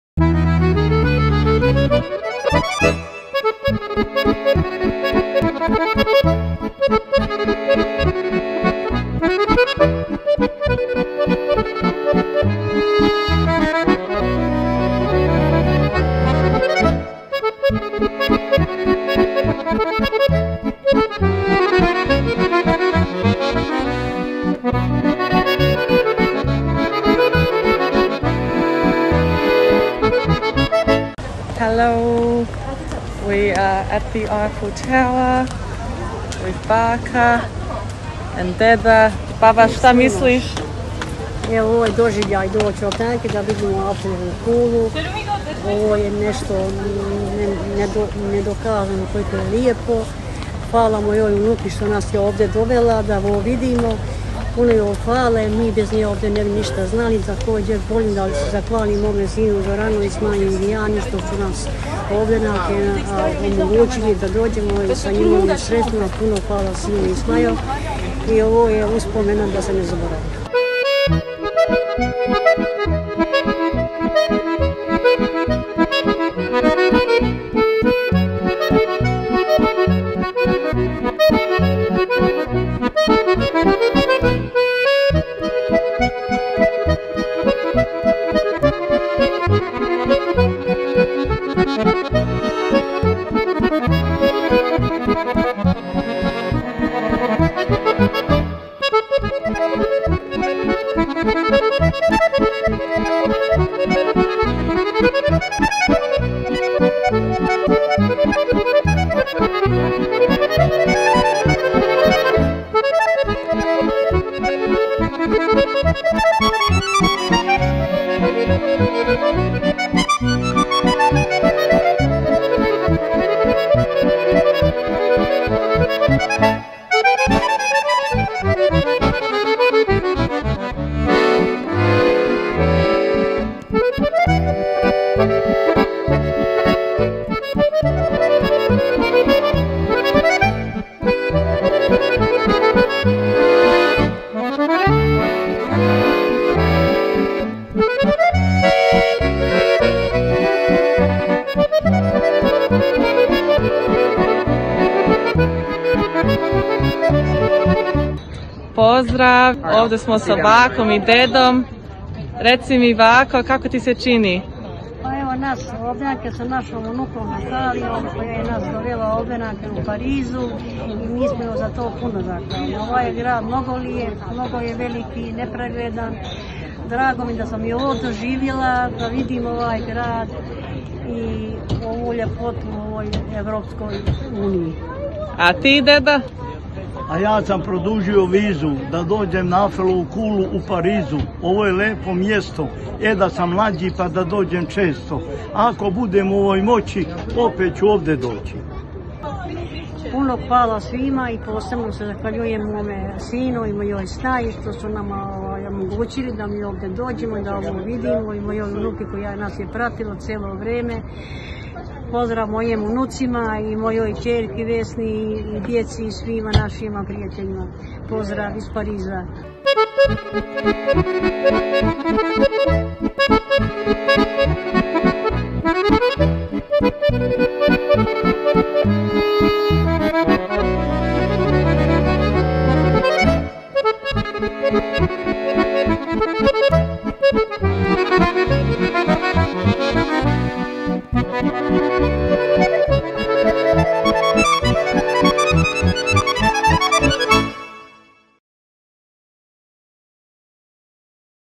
Pariz - Luka i Radojka Rakočević
Luka i Radojka Rakočević sa oduševljenjem posetom Francuske obišli i Ajfelovu Kulu u Parizu.